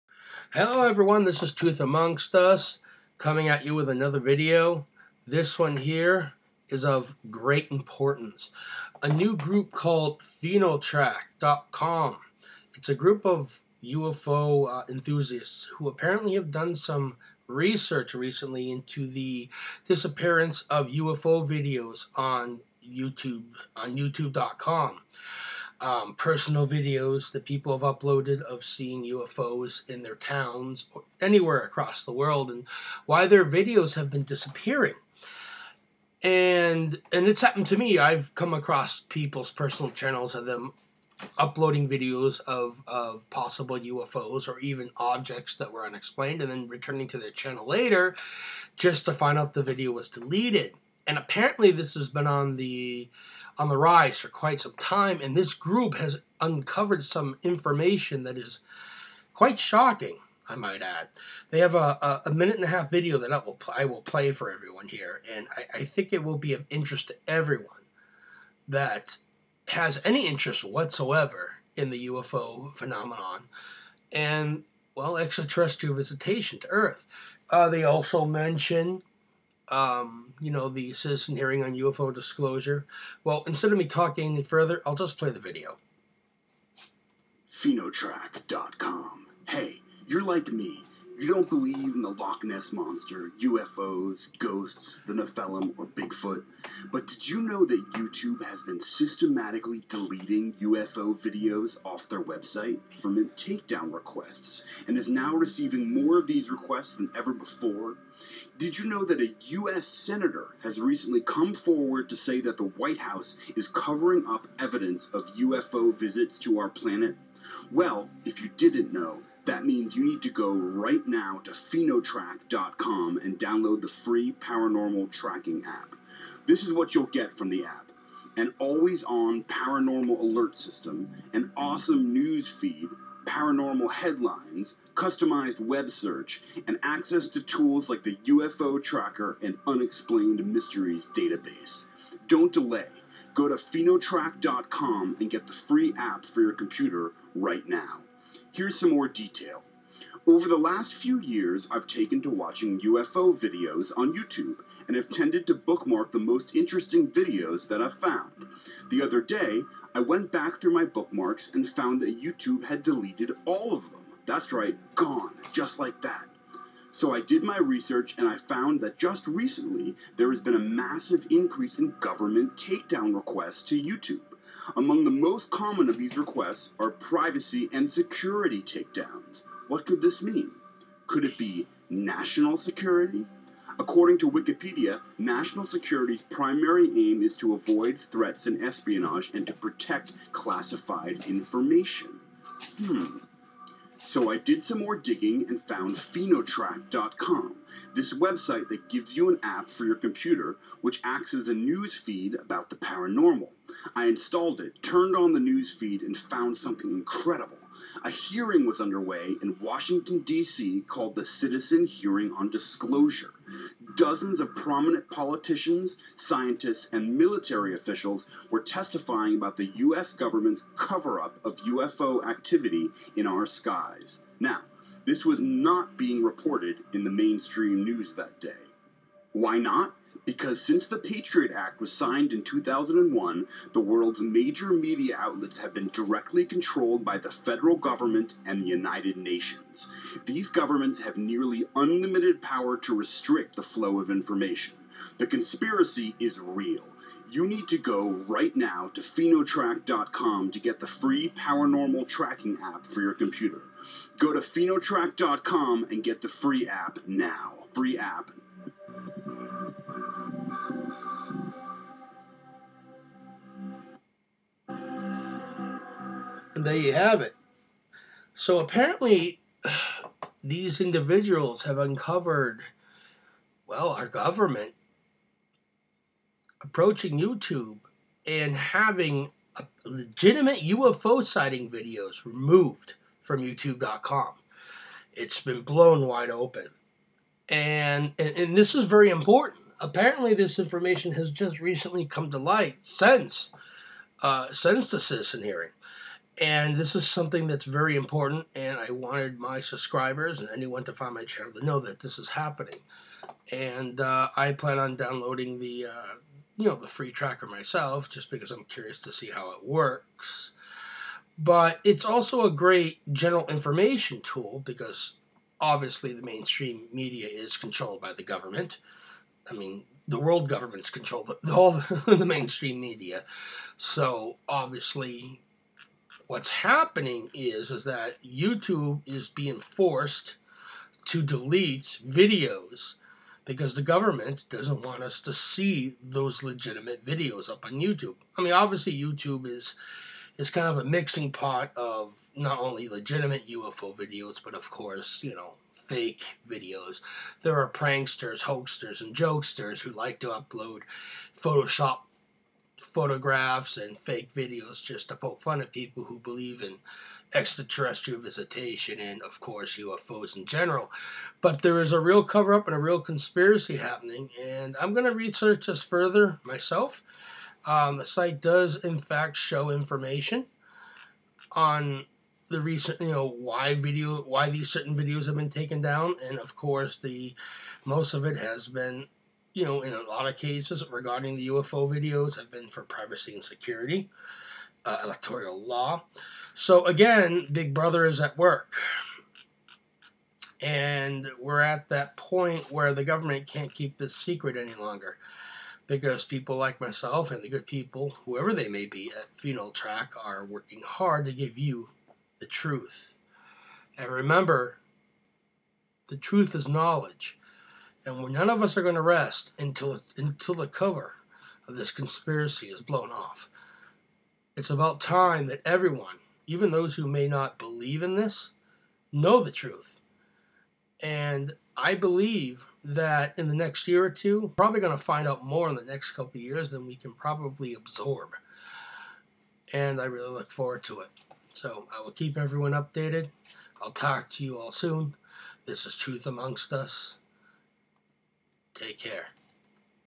Proof the US Government is Forcing Youtube To Take Down Legitimate UFO Videos?
Apparently the US Government has been forcing Youtube to delete legitimate UFO videos from its users' channels.